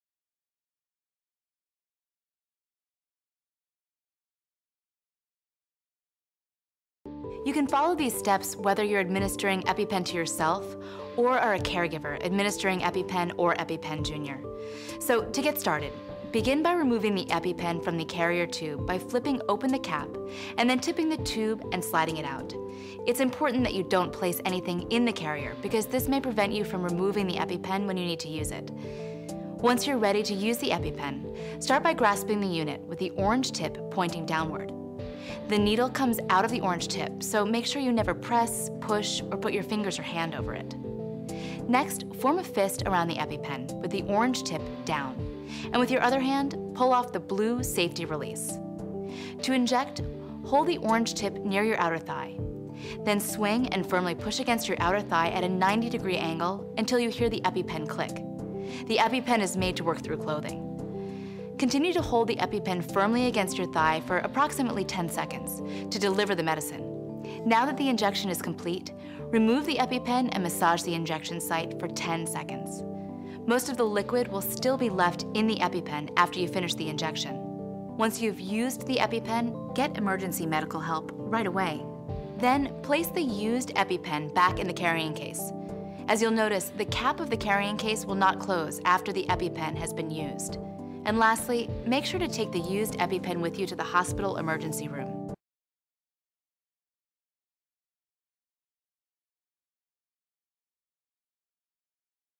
BFCCPS Epi-Pen Mid Year Training
Each year the BFCCPS nurses offer EpiPen training for the staff to able to administer epinephrine via auto-injector to a student with a previous diagnosis of a life-threatening allergy who is experiencing an anaphylactic reaction. In compliance with the state requirements we also provide you with a mid-year review for this content. All who participated in the annual EpiPen Training in the fall are receiving this email and are asked to complete the review prior to February vacation.